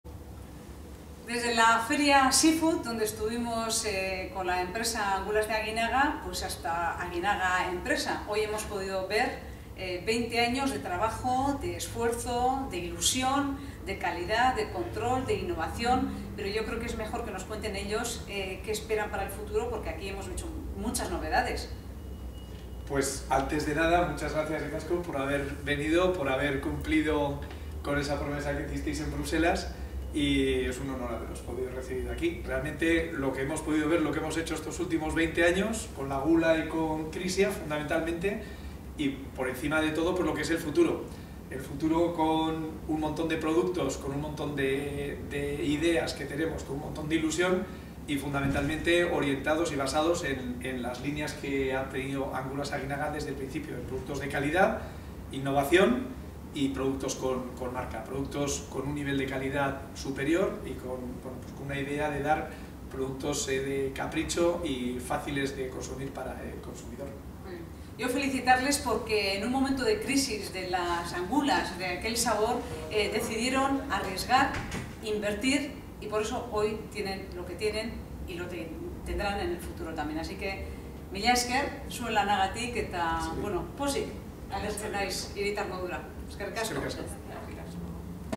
Calidad e innovación en angulas de Aguinaga
Con unos días de retraso quiero compartir con vosotros la experiencia que vivimos el diez de noviembre en la empresa Angulas de Aguinaga. No he podido subir esta crónica hasta hoy porque un problema técnico me impidió recuperar el vídeo que habíamos grabado en Irura y porque esta ha sido semana de pleno en la que apenas hay tiempo para nada. Hoy con un poco de ayuda he conseguido leer la tarjeta de la cámara y gracias a eso podéis ver aquí al grupo que nos explicó qué es hoy Angulas de Aguinaga: Johana Truyo, Juan Ignacio Azpiroz, Maddi Barandiaran y Jesús Alaba, nuestros guías en la visita.